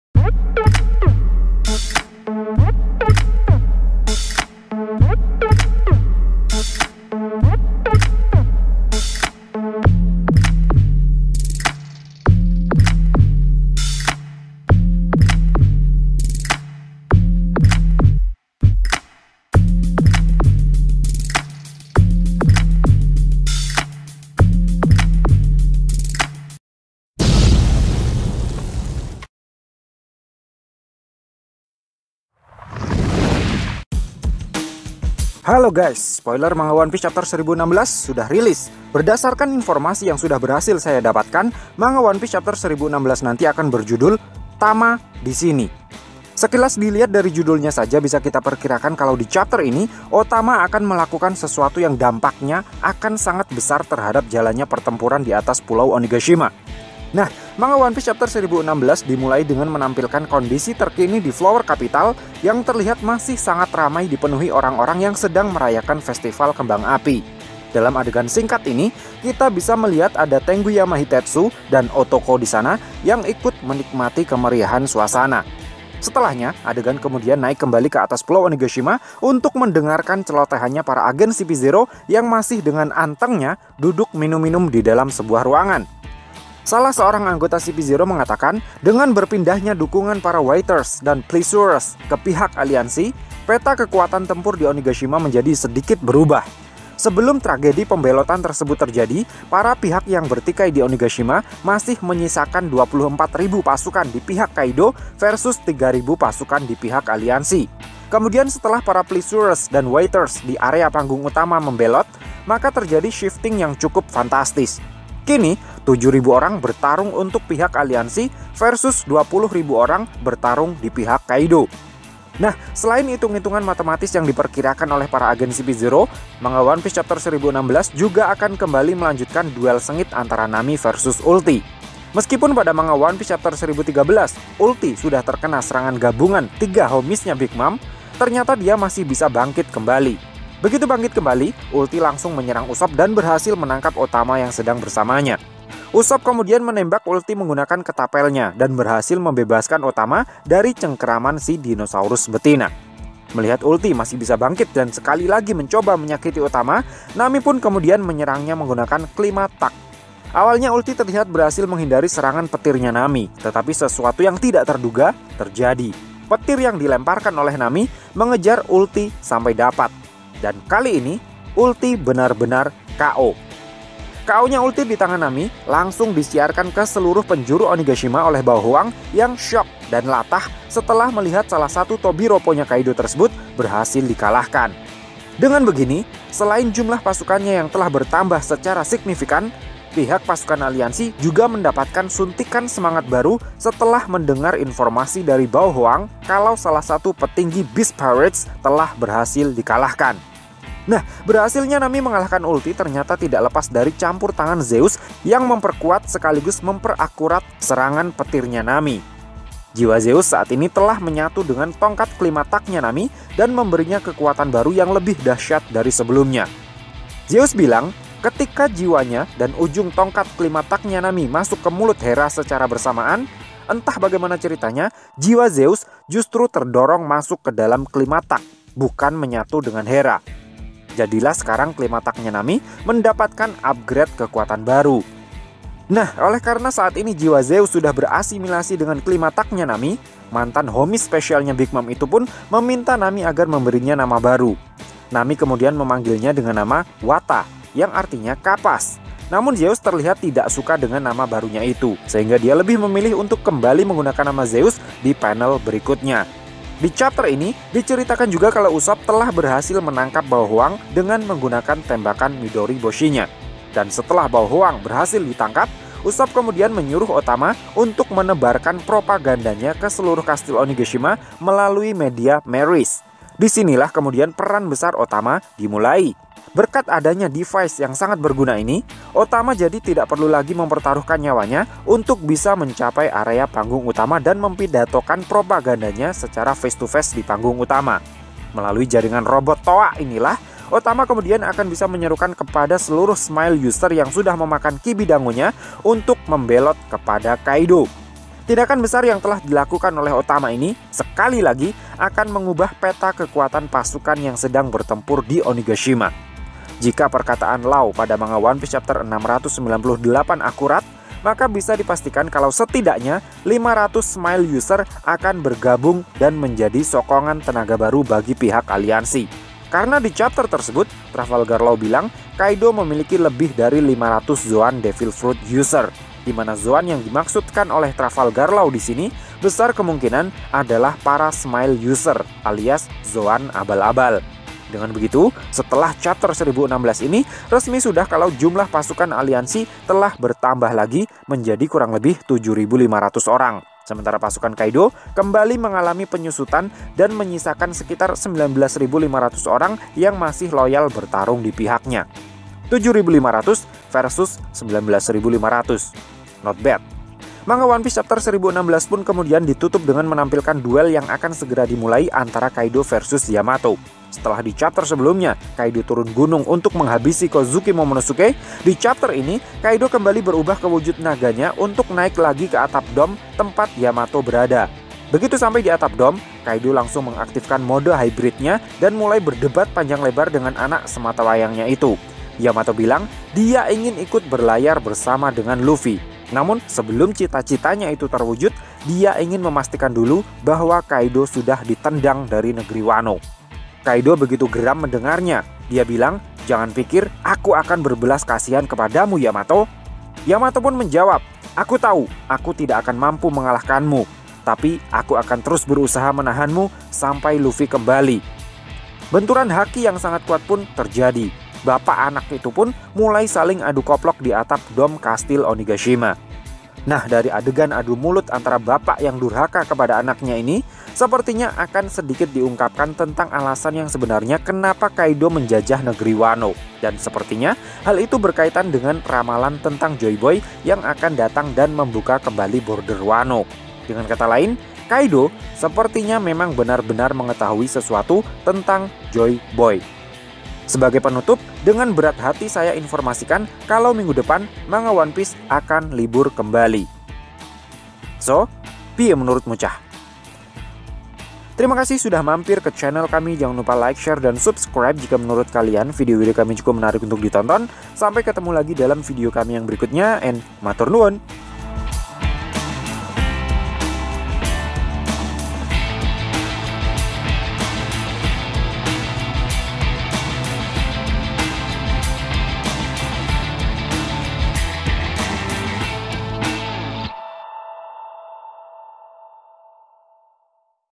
[ BOCORAN OP 1016 ] Kru Topi Jerami Paling Badass Adalah.... NAMI!!!
Spoiler One Piece Chapter 1016 By Real Gue Noe:
Kru Topi Jerami Paling Badass Adalah. NAMI!!!

Thanks To : Eiichiro Oda | Shueisa | Toei Animation